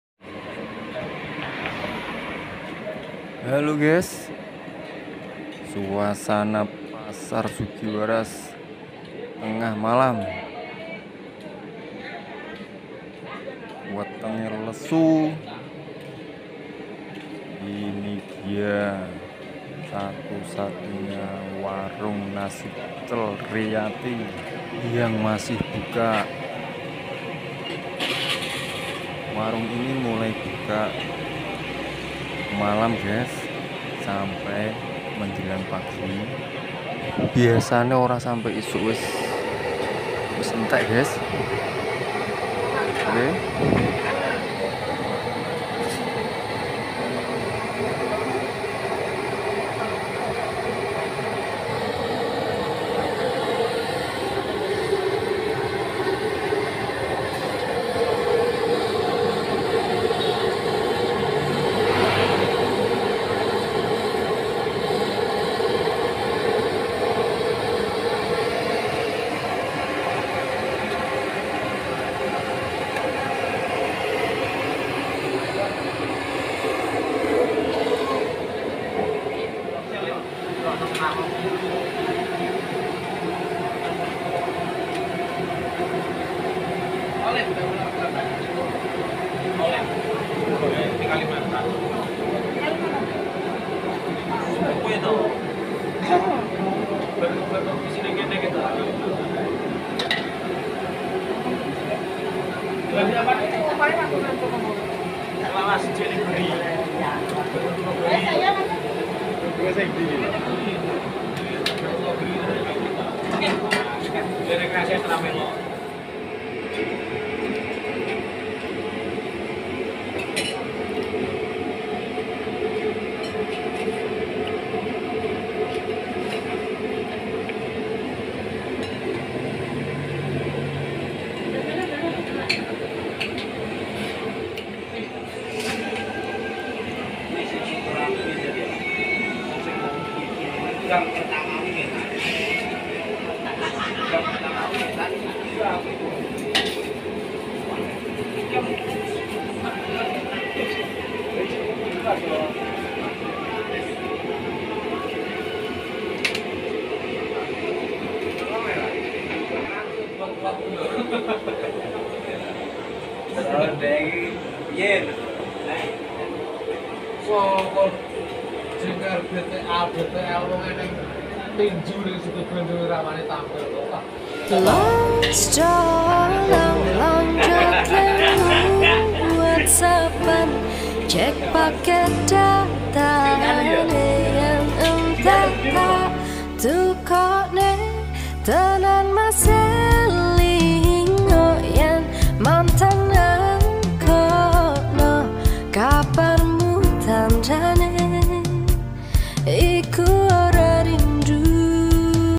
NASI PECEL RIYATI BUKA MALAM HARI DEPAN PASAR SUGIHWARAS
nasi pecel riyati warung legendaris turun temurun sejak tahun 90an sampe sekarng masih tetap ramai pengunjung, warung yang buka pukul 5 sore sampe malam ini tidak sepi pengunjung karena cuma satu-satunya warung nasi pecel yang buka malam hari.